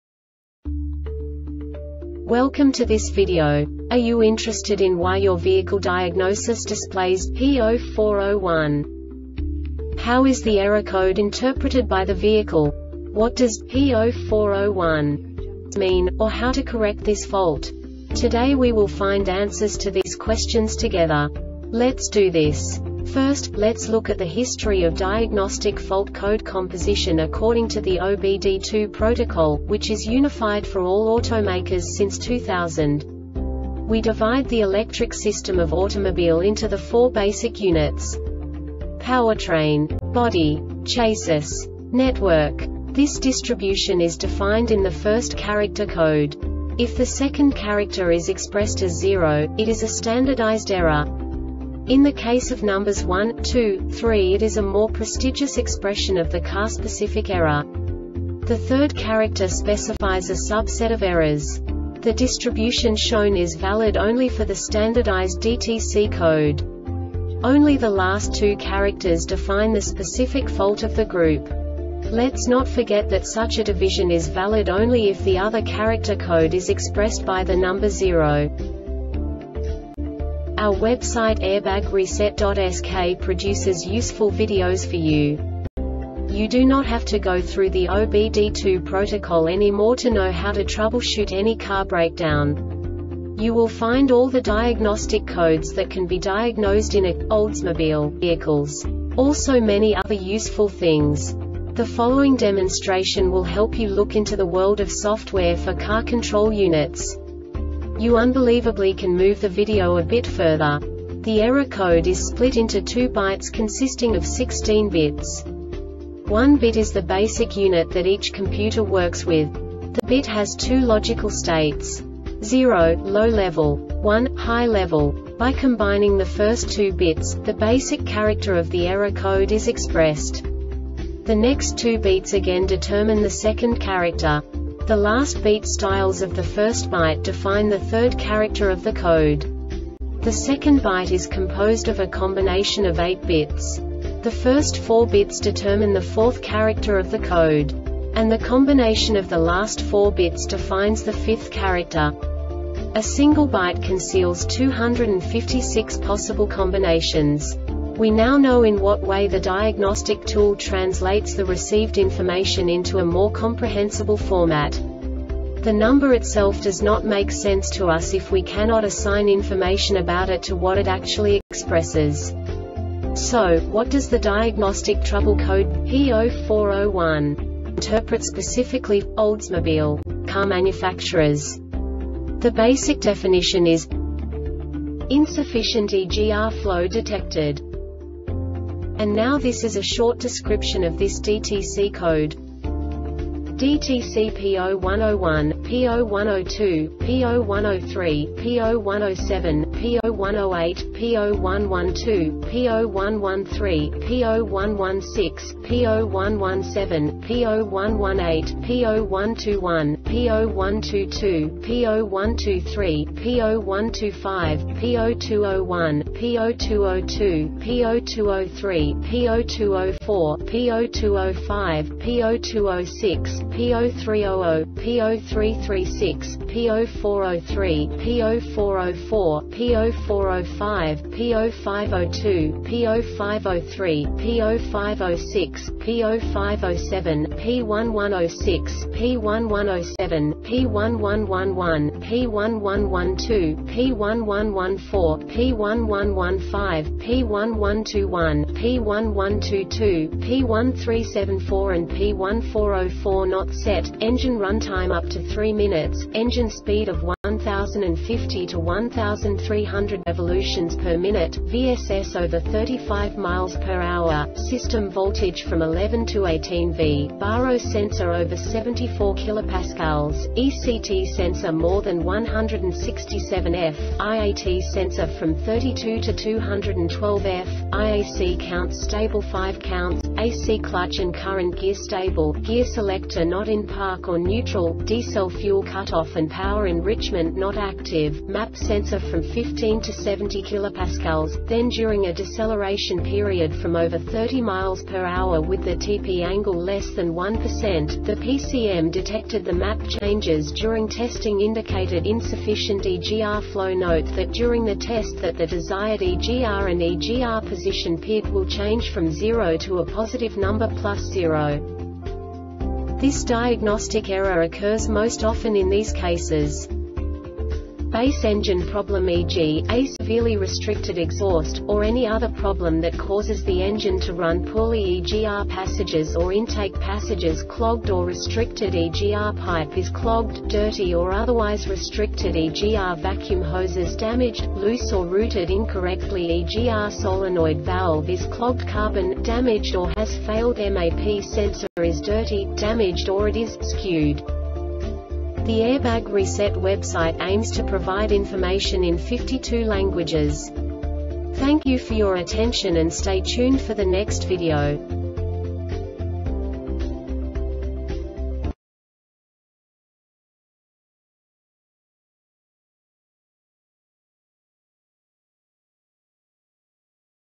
DTC Oldsmobile P0401 Short Explanation
Contents:
0:21 Basic DTC analysis according to OBD2 protocol standard.
1:48 Insight into programming
2:58 A diagnostically understandable description of the Diagnostic Trouble Code
3:05 Basic error definition

Make
Oldsmobile
Code
P0401
Definition
Insufficient EGR Flow Detected
Description
DTC P0101, P0102, P0103, P0107, P0108, P0112, P0113, P0116, P0117, P0118, P0121, P0122, P0123, P0125, P0201, P0202, P0203, P0204, P0205, P0206, P0300, P0336, P0403, P0404, P0405, P0502, P0503, P0506, P0507, P1106, P1107, P1111, P1112, P1114, P1115, P1121, P1122, P1374 and P1404 not set,  engine runtime up to 3 minutes,  engine speed of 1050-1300 rpm,  VSS over 35 mph,  system voltage from 11-18v,  BARO sensor over 74 kPa,  ECT sensor more than 167ºF,  IAT sensor from 32-212ºF,  IAC counts stable  5 counts,  A or C Clutch and current gear stable,  gear selector not in Park or Neutral,  Decel Fuel Cutoff and Power Enrichment not active,  MAP sensor from 15-70 kPa,  then during a deceleration period from over 30 mph with the TP angle less than 1 percent,  the PCM detected the MAP changes during testing indicated insufficient EGR flow  Note that during the test that the Desired EGR and EGR Position PID will change from 0 to a positive number + 0
Cause
Base engine problem e g ,  a severely restricted exhaust,  or any other problem that causes the engine to run poorlyEGR passages or intake passages clogged or restrictedEGR pipe is clogged,  dirty or otherwise restrictedEGR vacuum hoses damaged,  loose or routed incorrectlyEGR solenoid valve is clogged carbon,  damaged or has failedMAP sensor is dirty,  damaged or it is "skewed"